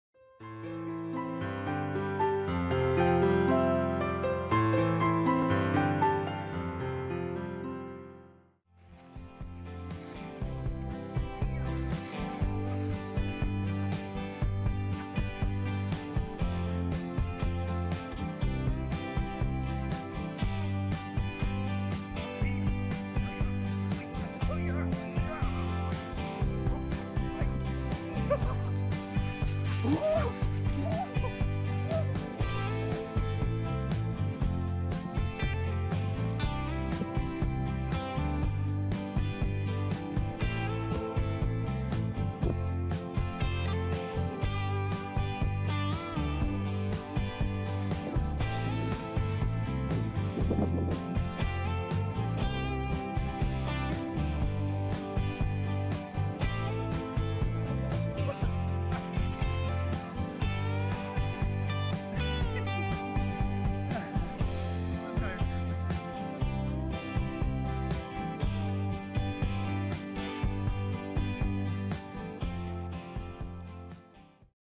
Standard Poodles Are High Energy and Playful!
"Are poodles high energy dogs?" YES!! Lacey is a very energetic standard poodle. Watch as she runs circles around her pack leader, Dan.

She also loves companionship with her 2 and 4 legged friends.